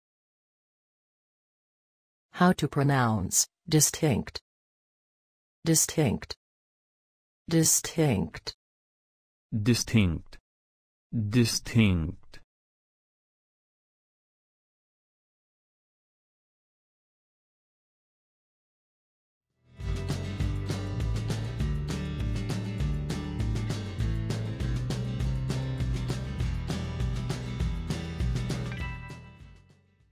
Distinct pronunciation
How to pronounce Distinct in English? Listen with us.
What is the correct pronunciation of the word Distinct in everyday English? Speak as the Americans.
How to write the word Distinct? Watch and learn.

Learn how to pronounce Distinct.